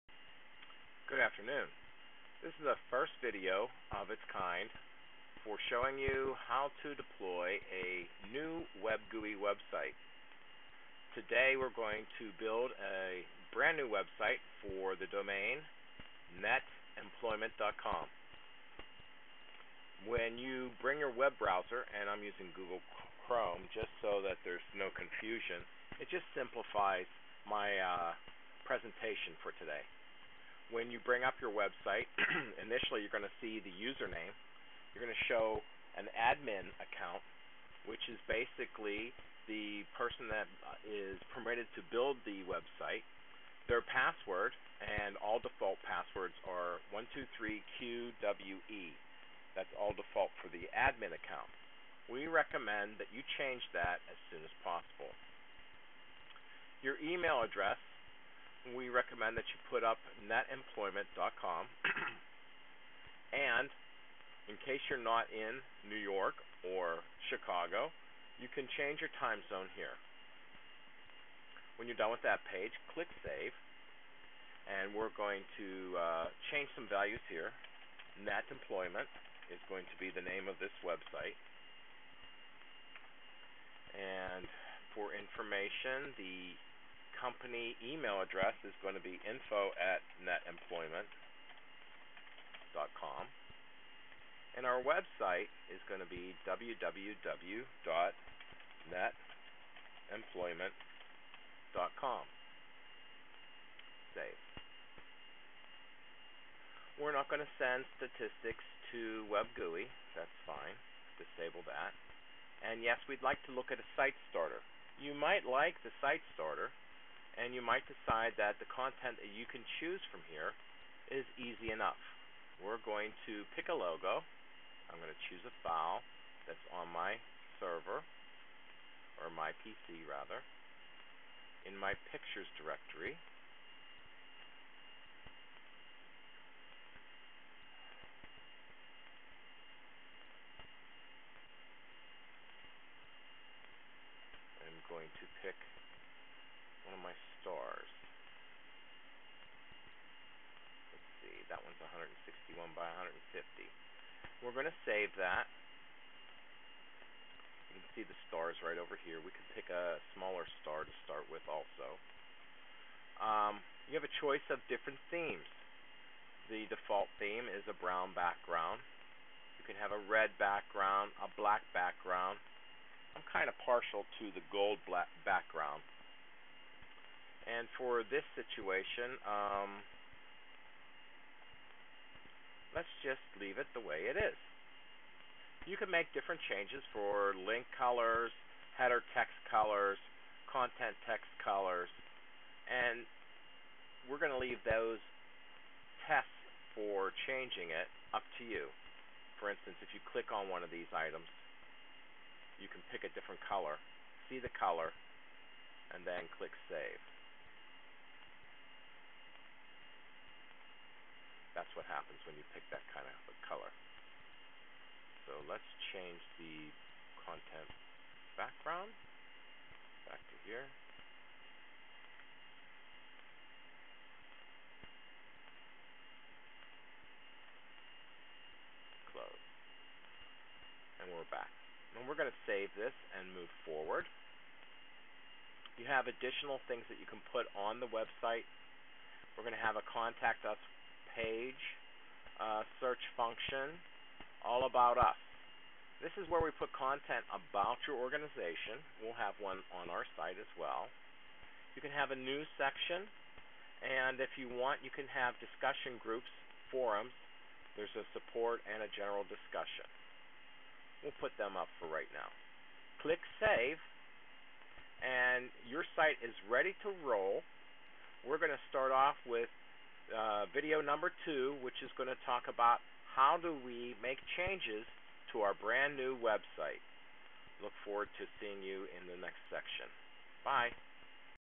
New WebGUI website.mp4
This videos shows how simple and easy it is to create a New Website using the Hosting Services @ ABS Computer Technology, Inc.   This video displays how the My Site features in our WebGUI Hosting service enable you complete control when you create the first website.